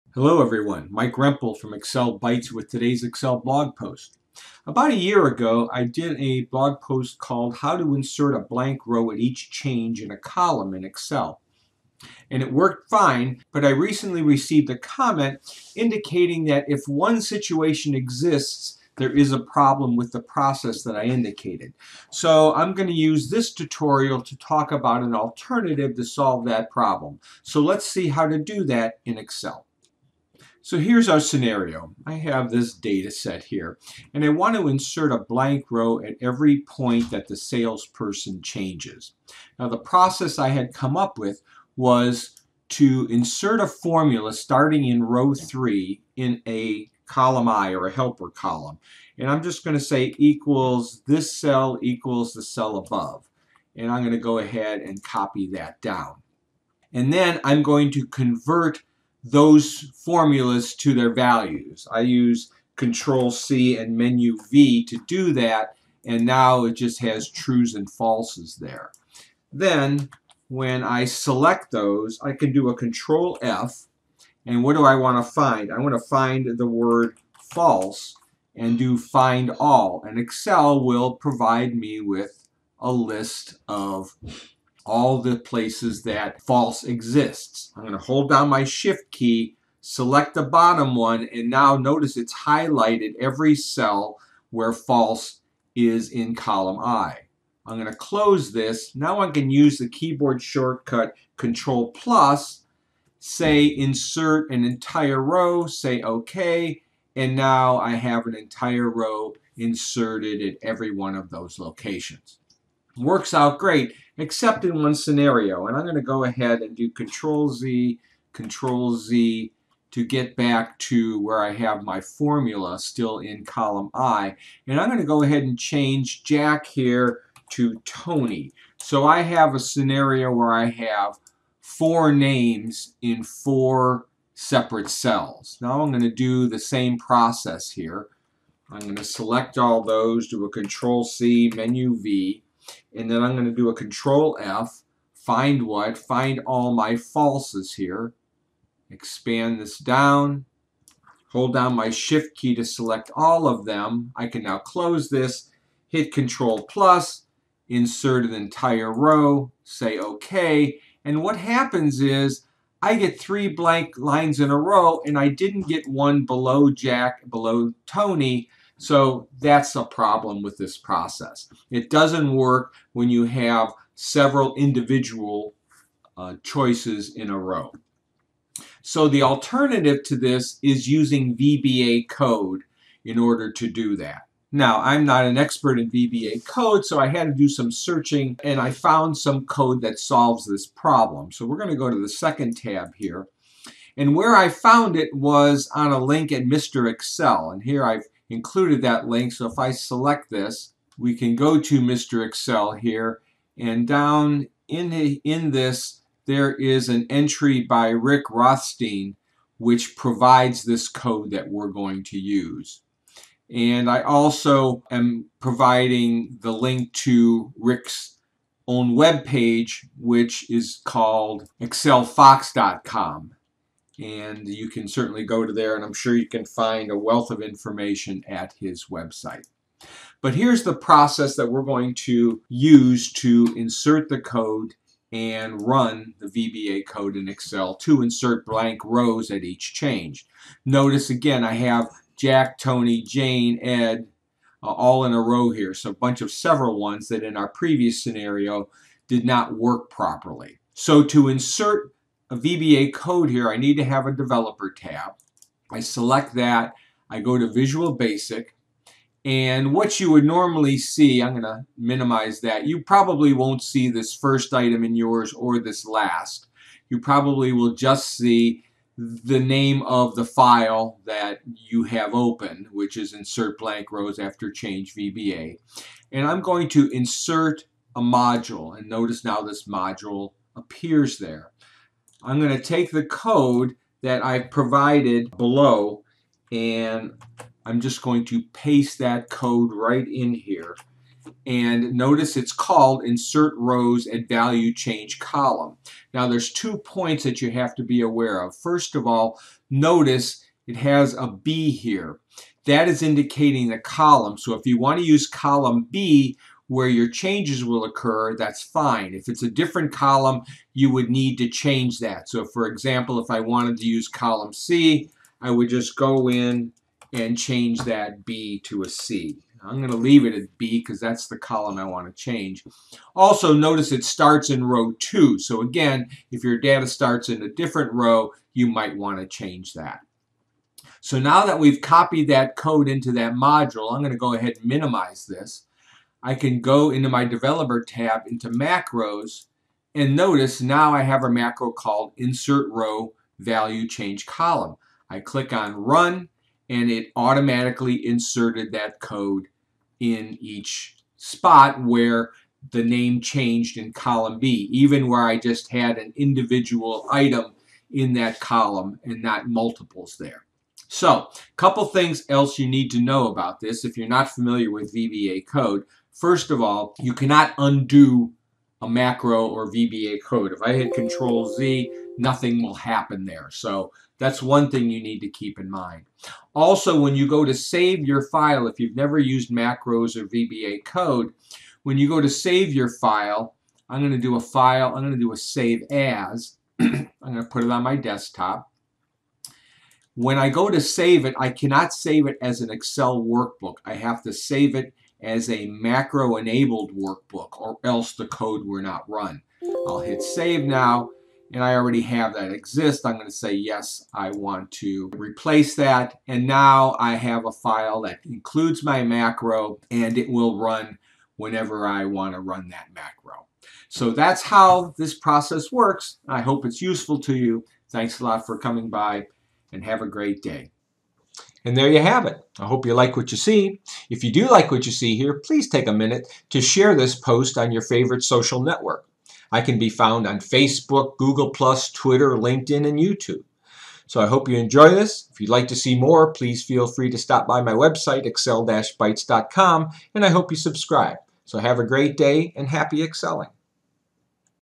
Inserting A Blank Row At A Column Change UPDATE In Excel
In this tutorial we are going to look at an alternate way to insert a blank row when data changes in a column using VBA to accomplish that.

To get the VBA code, go to the site below or download the file as noted above and download it from the VBA Editor.